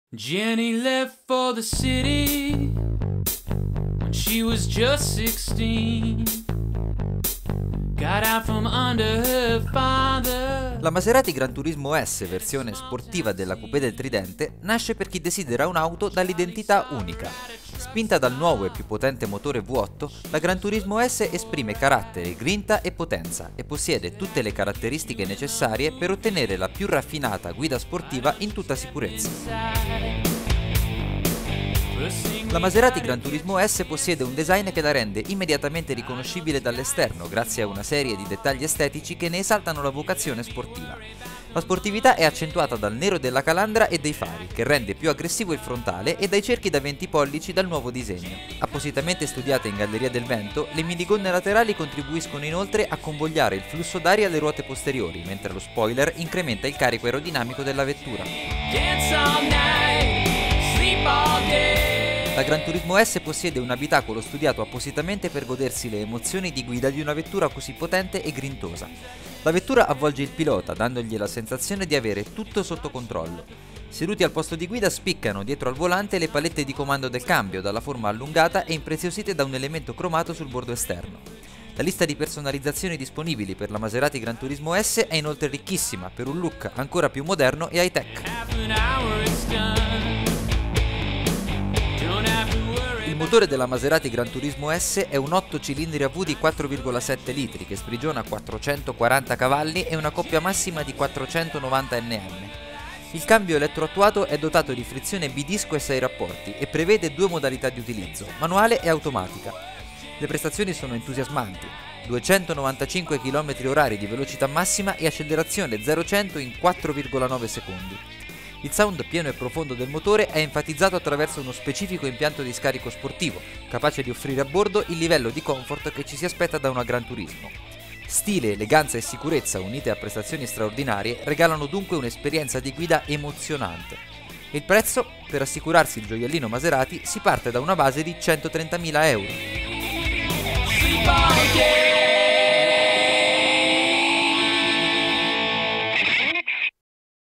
Maserati Granturismo S
La versione sportiva della coupé del tridente, con un motore V8 da 440 Cv.
Servizio di Manuel Codignoni